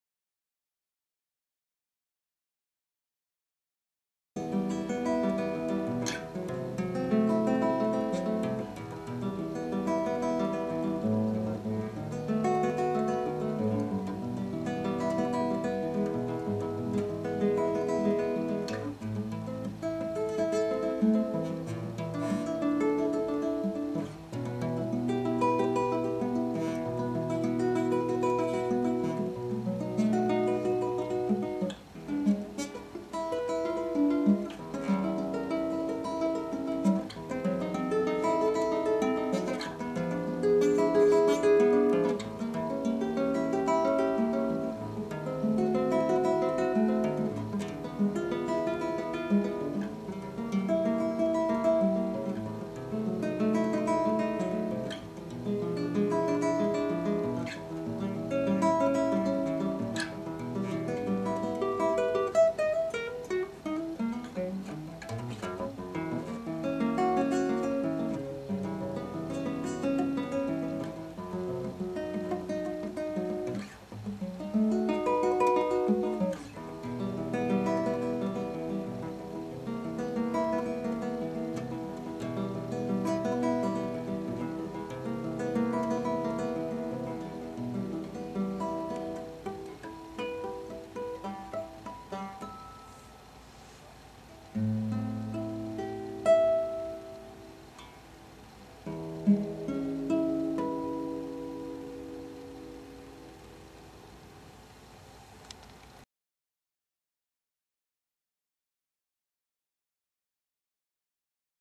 Chase Allen Jarvis on Classical Guitar
My good friend Chase Jarvis.  Some old footage of him playing some Classical Guitar,  his You Tube channel is linked at the end.